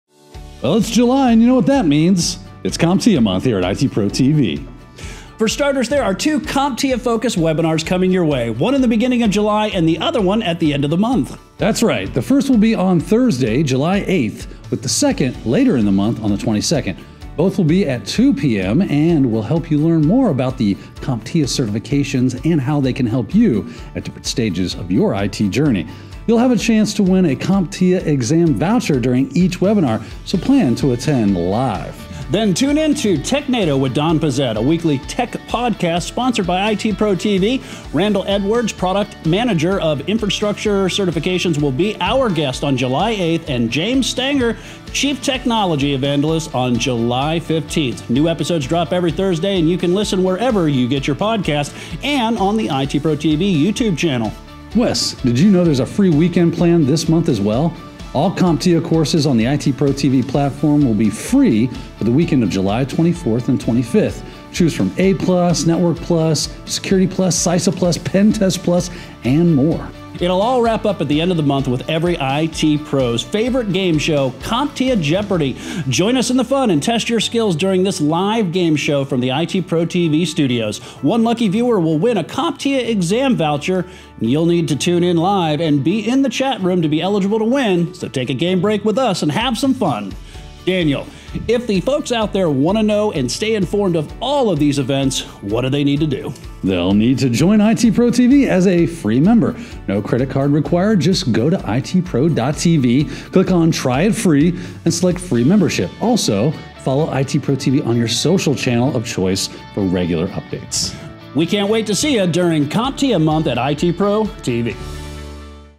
July 2021 is CompTIA Month at ITProTV - FREE Exam Vouchers Giveaways
It's all about CompTIA this July at ITProTV. There will be 2 webinars dedicated to CompTIA certifications, as well as a couple of guests on Technado with Don Pezet, the weekly tech news podcast. The month will end with CompTIA Jeopardy. There will also be four CompTIA exam voucher giveaways, so make sure you tune in live for a chance to win!

Save these dates in your calendar:

July 8th, 2pm ET
Webinar #1: Explore Next-Level CompTIA Cybersec Certs

July 22nd, 2pm ET
Webinar #2: First Look at the Updated CompTIA CASP+

July 24th-25th
CompTIA Free Weekend

July 30th, 2pm ET
Gameshow: CompTIA Jeopardy